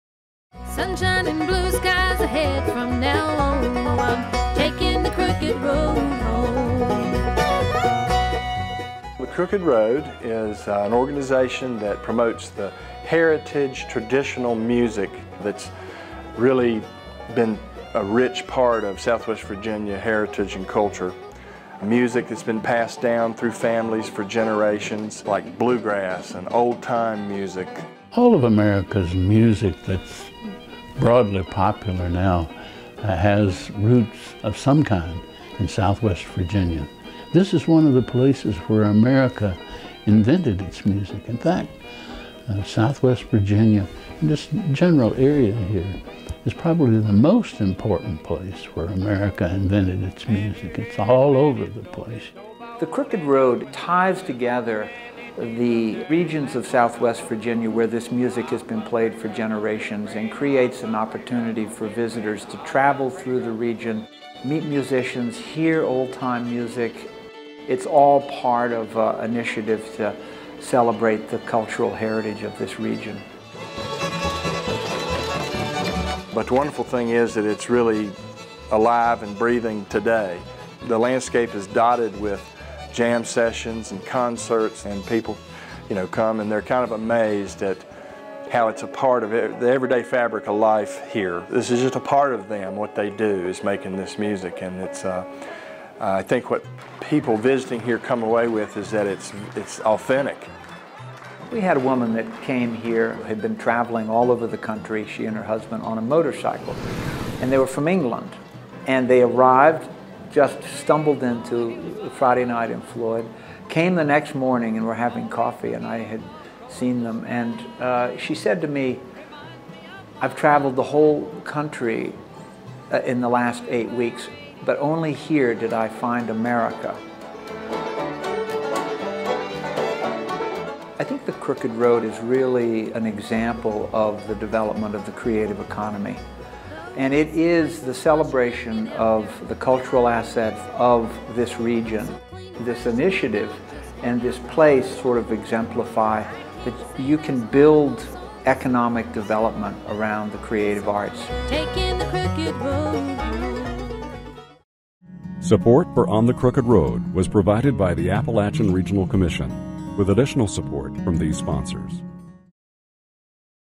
On The Crooked Road - Overview
What is The Crooked Road? How did it come about? The Crooked Road, Virginia’s Heritage Music Trail, winds over 300 miles through the mountains of Southwest Virginia. Connecting major music venues, music jams, and festivals in communities all along the way,